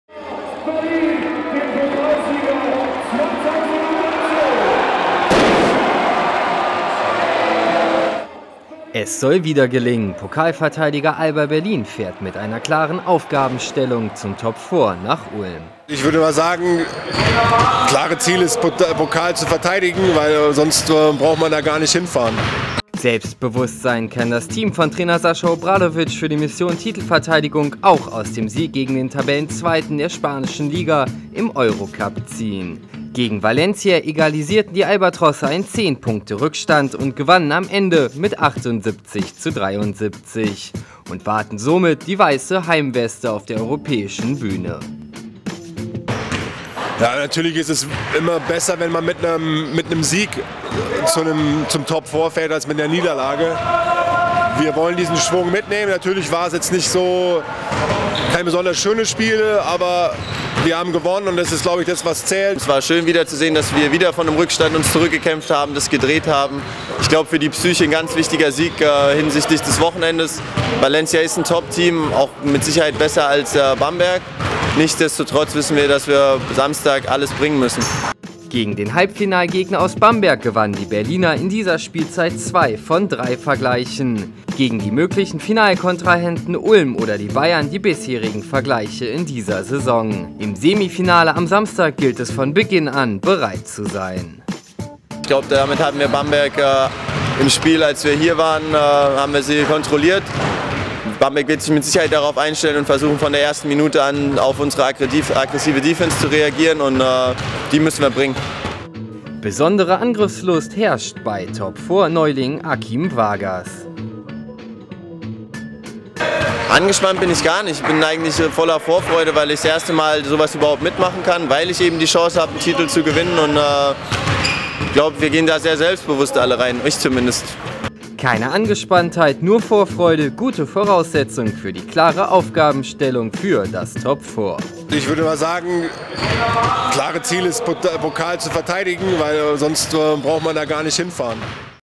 Alba unter den Top Four
Das Ziel ist klar, Alba Berlin will den Pokal verteidigen. Dass dies eine schwere Aufgabe ist, zeigt die Tabelle, denn beim Top Four treten am Wochenende in Ulm die ersten Drei der Liga, plus Gastgeber an. Warum der Tabellendritte allerdings gute Chance hat, den Titel zu verteidigen, zeigt dieser Beitrag.